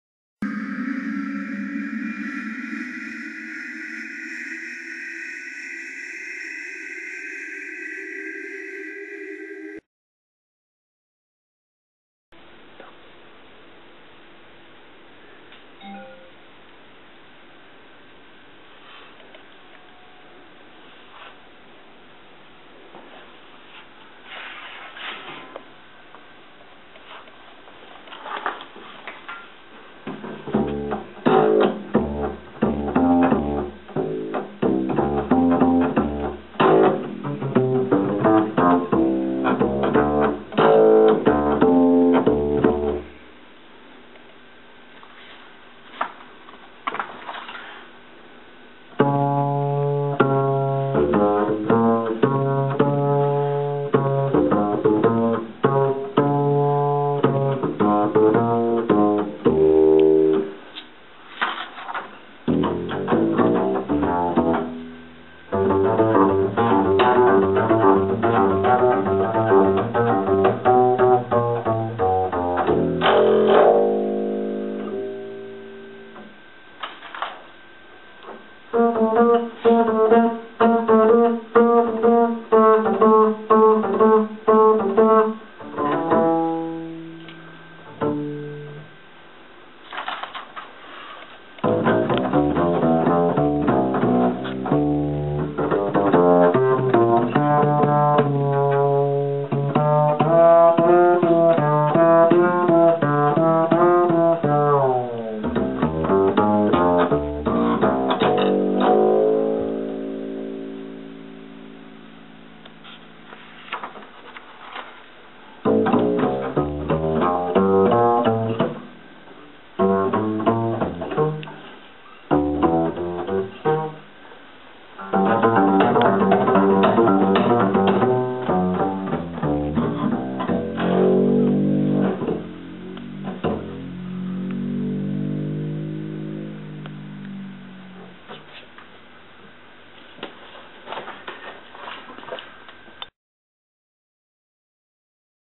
ACTE n°12 : Musique de Base
Célestin Zèbre inaugure la rubrique « Musique de Base » de manière stupéfiante ! En effet, délaissant son habituel théorbe, il s'est emparé d'une contrebasse [...]